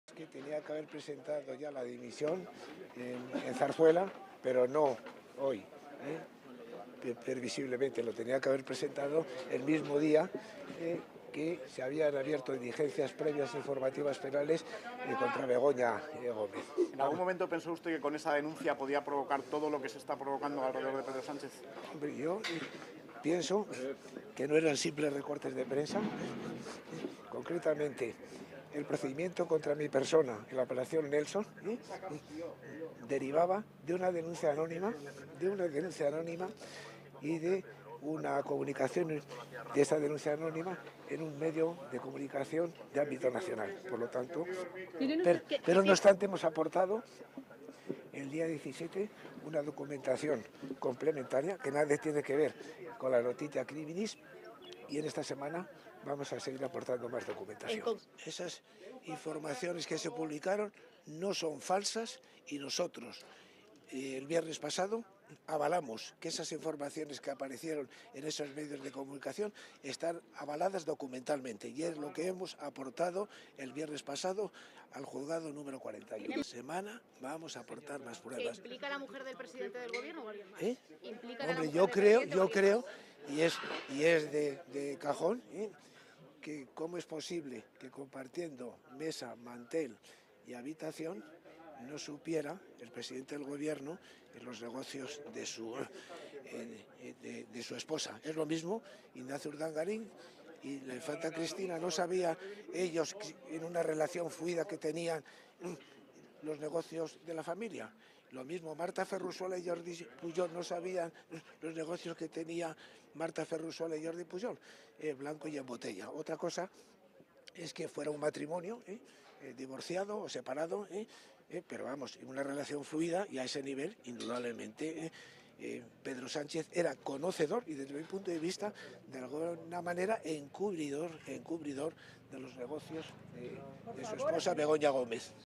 El Caso Begoña Gómez va pa' largo
La investigación a Begoña Gómez, esposa del presidente del Gobierno, entró el miércoles en una segunda fase al marcar los jueces de la Audiencia Provincial hasta donde alcanza la denuncia de Manos Limpias para investigar. Y de todos los supuestos indicios presentados por el pseudosindicato solo se salvan las dos copias de una carta-tipo en la que Begoña Gómez estampó su firma recomendando a una UTE de empresas que finalmente consiguieron dos contratos públicos.